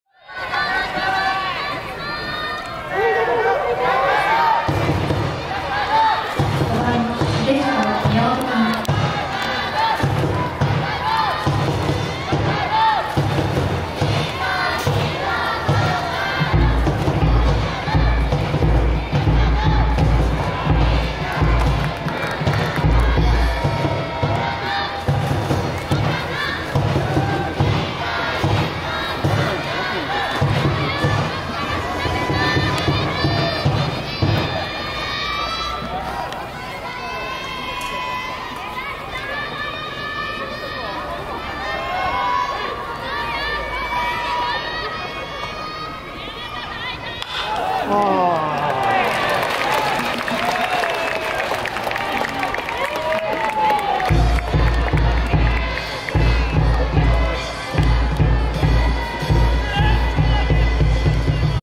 2023/9/27(水) 常廣羽也斗(青山学院大学) vs 宮本涼太(東洋大学) ライト前ヒット
東都大学野球 2023秋季リーグ戦(1部) 2023/9/27(水) 明治神宮野球場 青山学院大学 vs 東洋大学 1回戦

4回裏（東洋大学の攻撃）
（先発投手）常廣
2アウト 宮本 ライト前ヒット

【勝投手】常廣
【敗投手】岩崎
清水(球)、西村忠(一)、中野(二)、大槻(三)